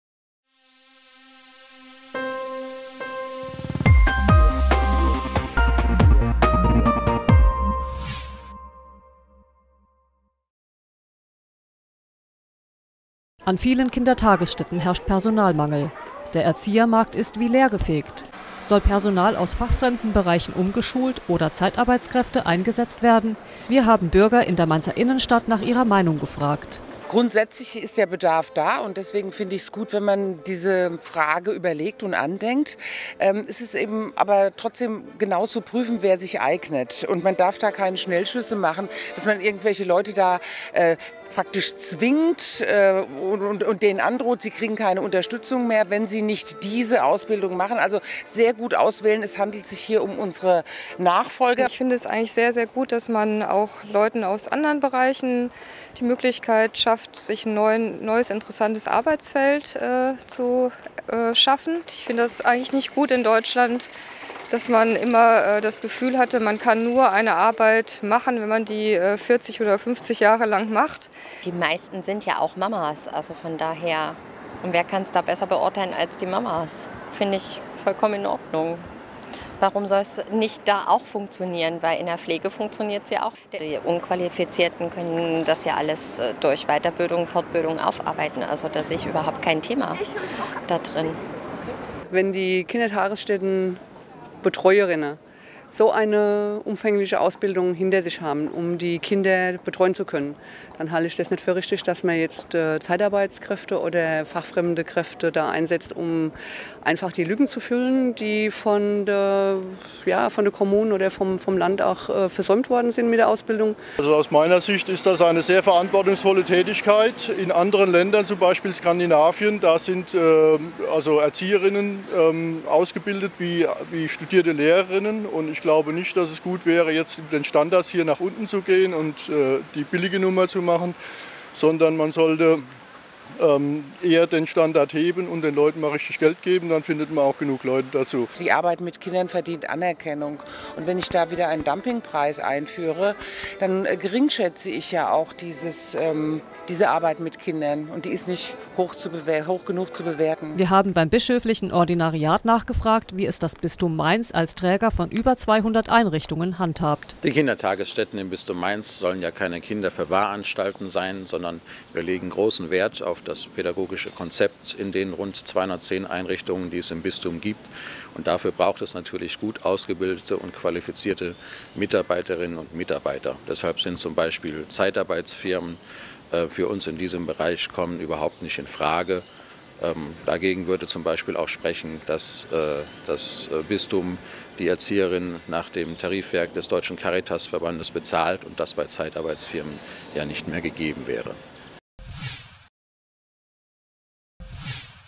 Mainz: Quereinsteiger in Kitas?
Den Kindertagesstätten fehlen Erzieher. Soll man fachfremdes Personal umschulen oder Zeitarbeitskräfte einsetzen? Wir haben uns in der Innenstadt umgehört und auch das Bistum Mainz um eine Position gebeten. Weitere Videos aus Mainz und Umgebung sehen Sie